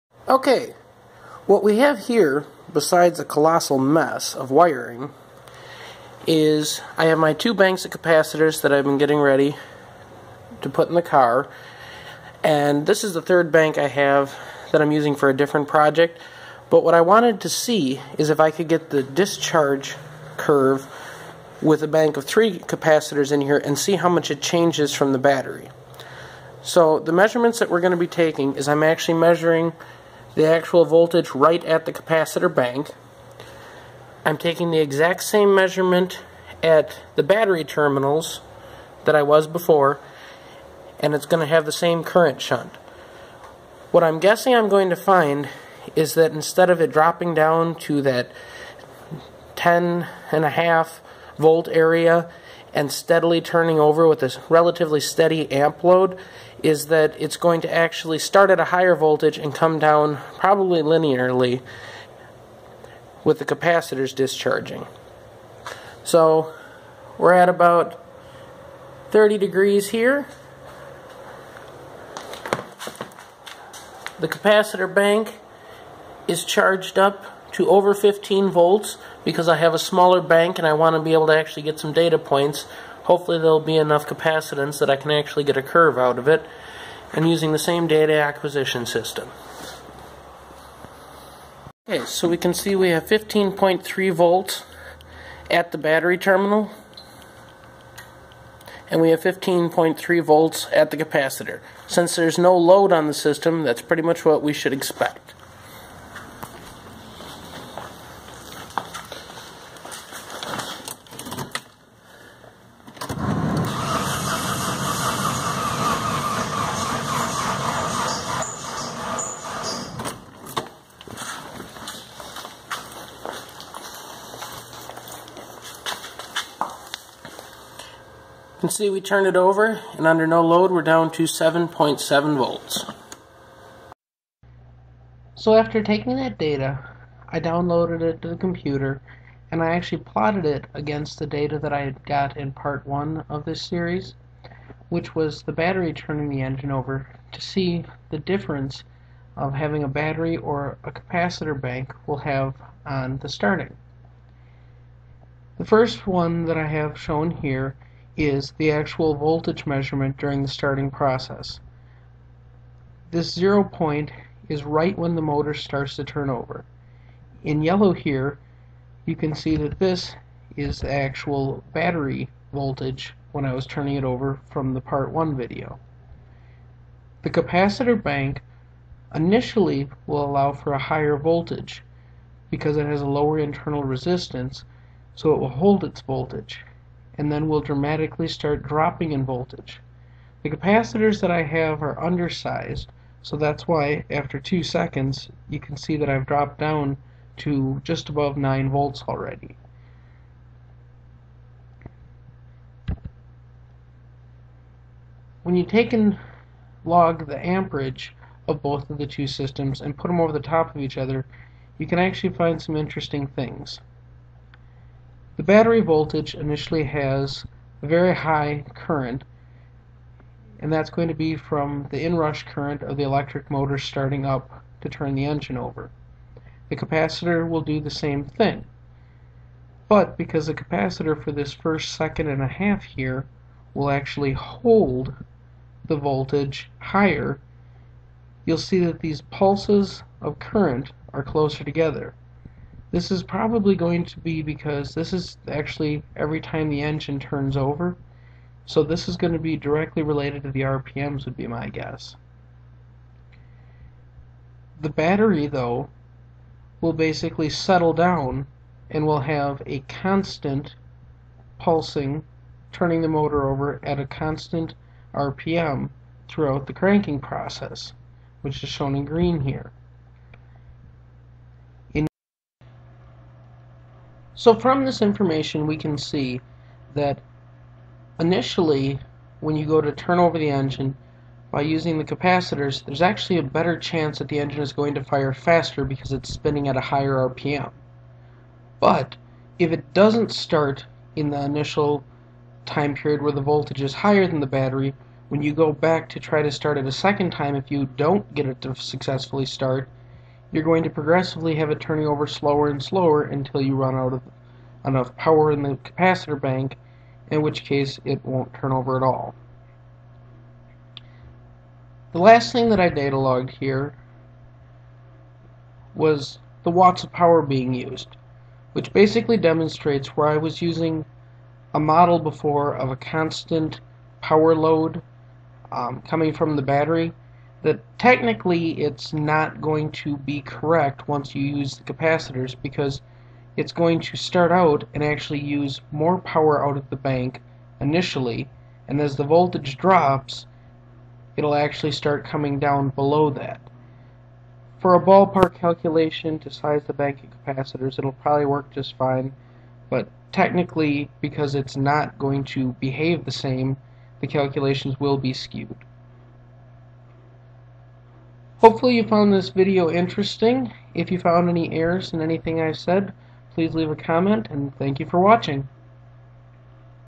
Using a super capacitor as a battery (part 4)
Using a super capacitor as a battery (part 4).  I turn over the engine with a 249 F of capacitors and compare the performance to the  battery.